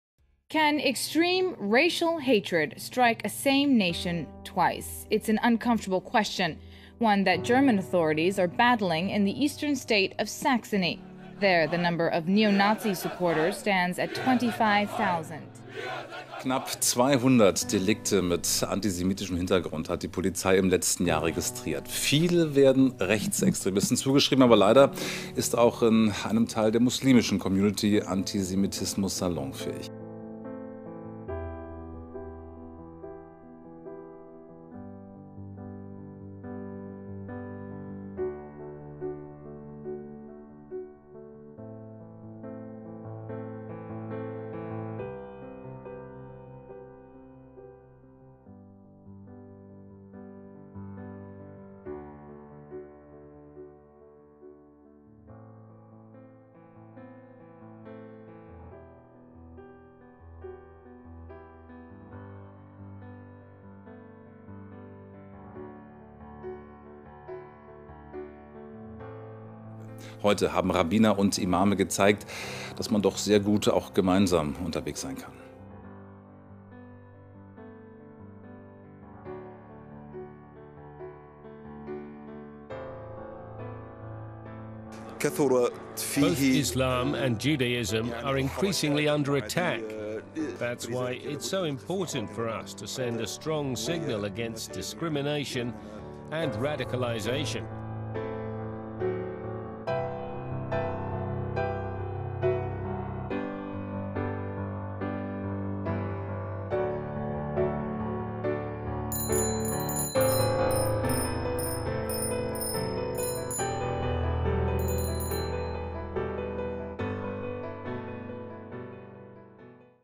Cycling Unites – The Unlikely Tandem
What all started with a poster idea for the Berlin Bicycle Week became a huge demonstration for tolerance and against extremism. Our message Cycling Unites together with the picture of an imam and a rabbi on a tandem became the icon and symbol for a movement.